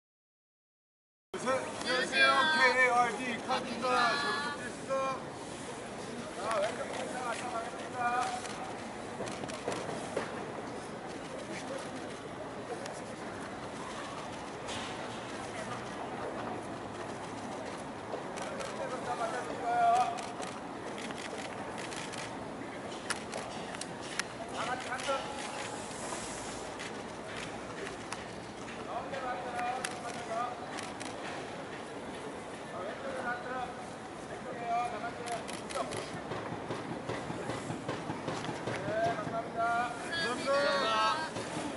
[뉴스인스타] 171201 카드(KARD) KBS 뮤직뱅크 출근길
171201 카드(KARD) 뮤직뱅크 리허설 출근길 풍경 입니다.

지구(GATE 9), 카드(KARD), NRG, 빅톤(VICTON), 구구단, 러블리즈, 블라블라(Blah Blah), 사무엘, 소나무, 아스트로(ASTRO), 이하린, 펜타곤, 원앤비, 마르멜로(MARMELLO), 유설, 민서, BLK, 해아(립버블)가 참여했습니다.